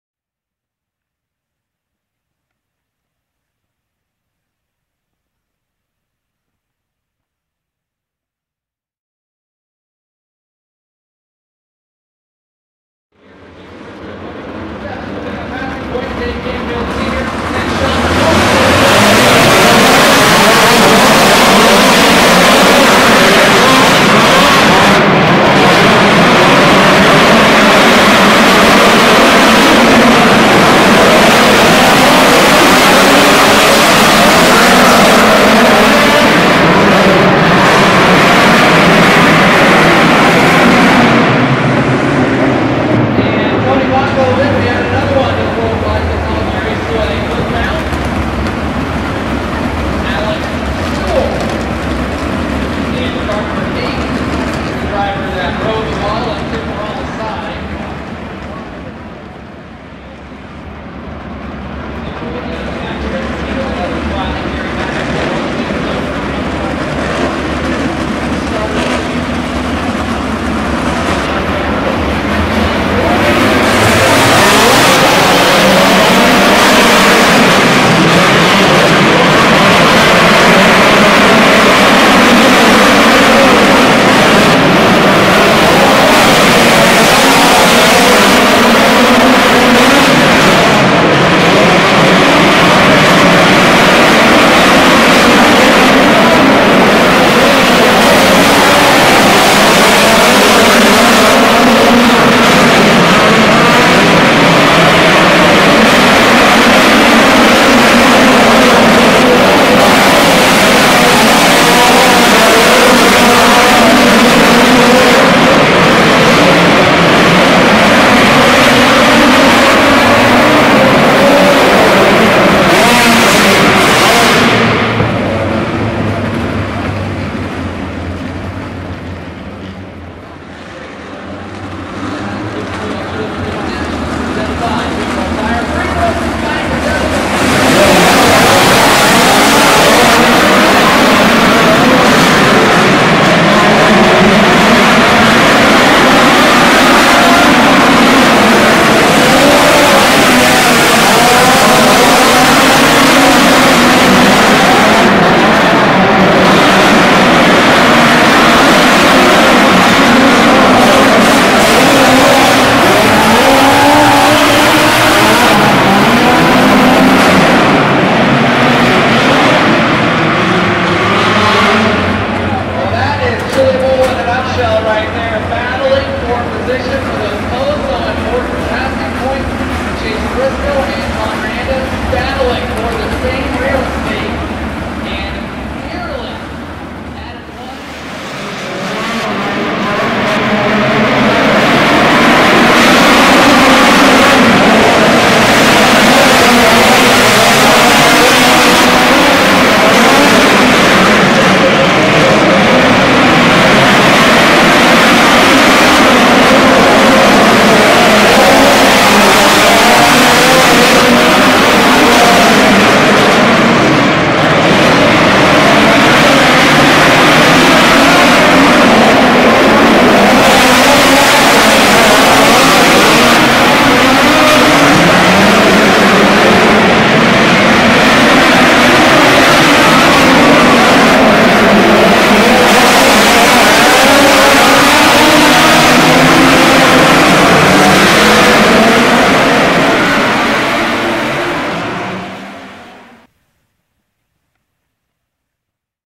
2020 Chili Bowl - Monday Heat 2
ChiliBowl2020 Monday Heat 2 (8 Laps): 1. 22S-Sean McClelland[2]; 2. 5-Chase Briscoe[7]; 3. 86C-David Camfield Sr[1]; 4. 47K-Kevin Brewer[4]; 5. 155-Ryan Truitt[3]; 6. 1B-Anton Hernandez[5]; 7. (DNF) 5F-Danny Frye III[8]; 8. (DNF) 8-Alex Sewell[6]